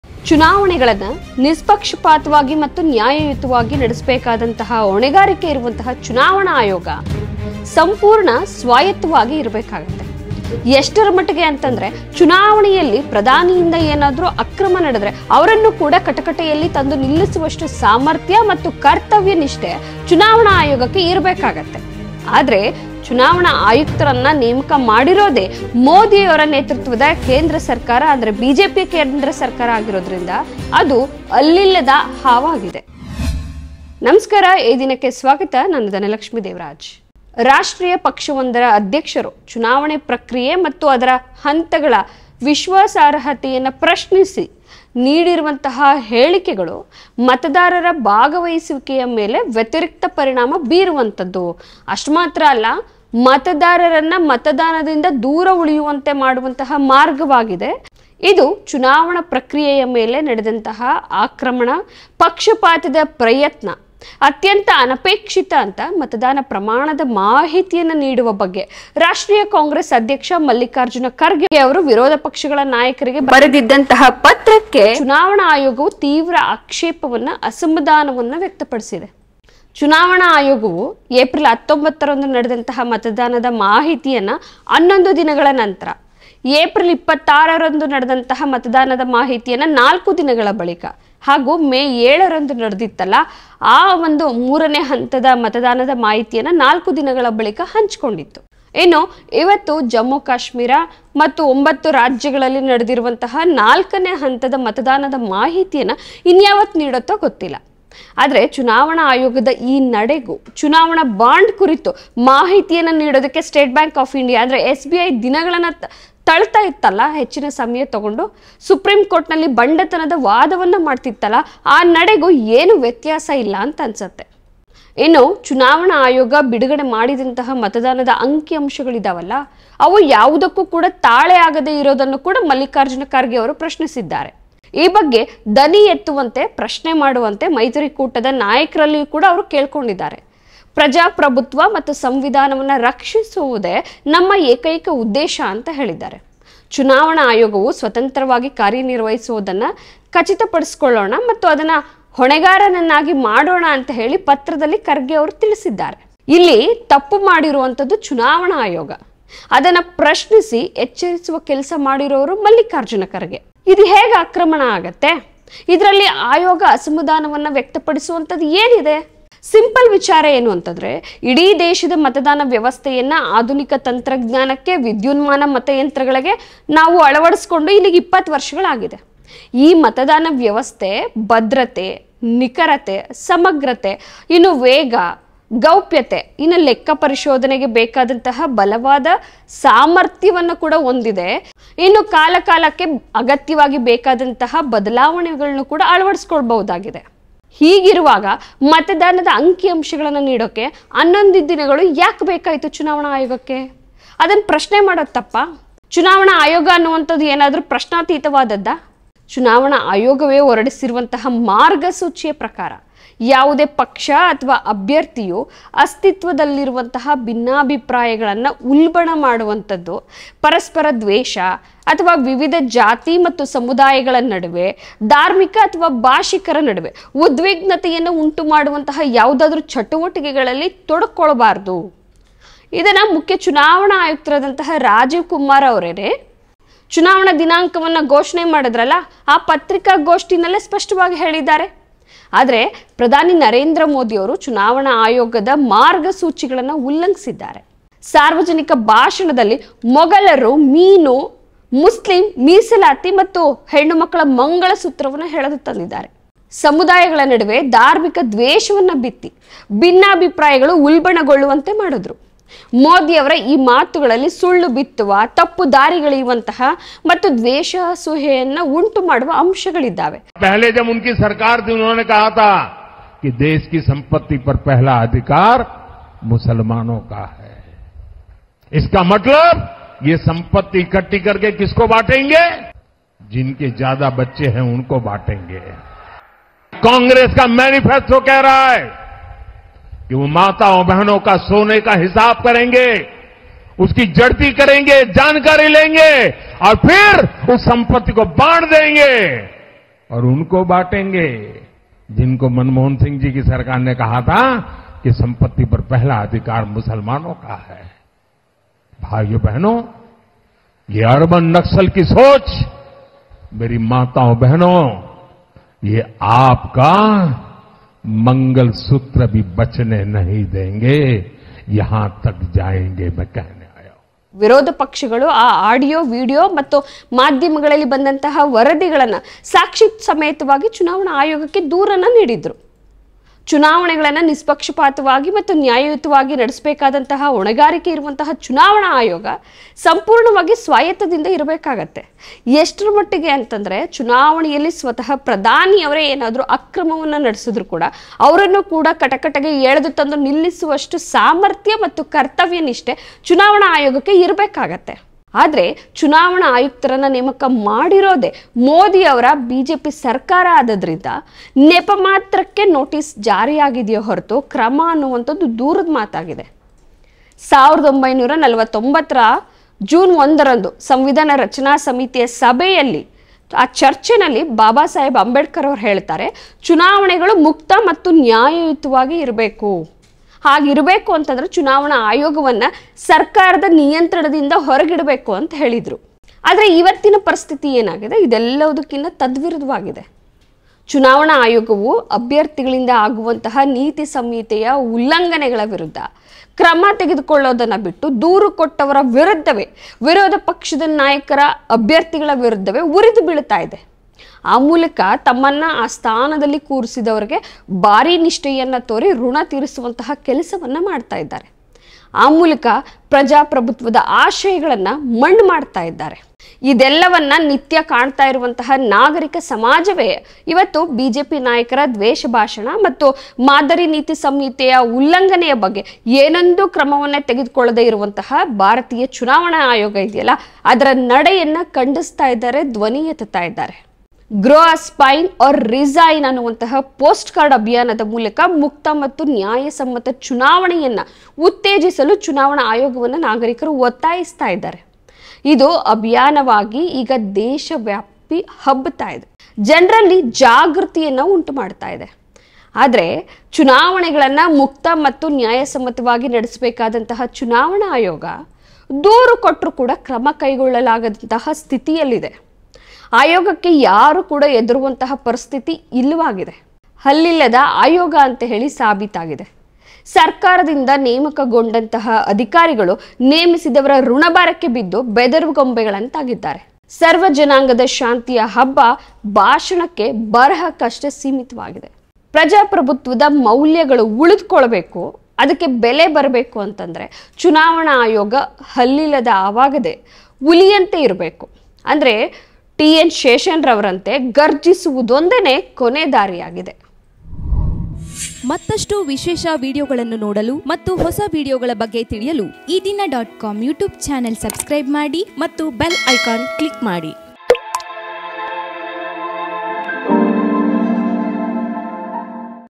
ಮೋದಿಯನ್ನೂ ಕಟಕಟೆಗೆ ನಿಲ್ಲಿಸುವುದೇ ಚುನಾವಣಾ ಆಯೋಗ?
ಚುನಾವಣೆಯಲ್ಲಿ ಪ್ರಧಾನಿ ಅಕ್ರಮ ನಡೆಸಿದರೂ ಅವ್ರನ್ನ ಕಟಕಟೆಗೆ ನಿಲ್ಲಿಸುವಷ್ಟು ಸಾಮರ್ಥ್ಯ- ಕರ್ತವ್ಯನಿಷ್ಠೆ ಚುನಾವಣಾ ಆಯೋಗಕ್ಕಿರಬೇಕು. ಆದರೆ, ಮೋದಿಯವರು ನಿರಂತರವಾಗಿ ದ್ವೇಷ ಭಾಷಣಗಳನ್ನ ಮಾಡ್ತಾ ಇದ್ರೂ, ಚುನಾವಣಾ ಆಯೋಗ ಕ್ರಮ ಕೈಗೊಳ್ಳುತ್ತಿಲ್ಲ.ಬದಲಿಗೆ ದೂರು ನೀಡಿದವರ ವಿರುದ್ಧವೇ ಉರಿದು ಬೀಳ್ತಾ ಇದೆ.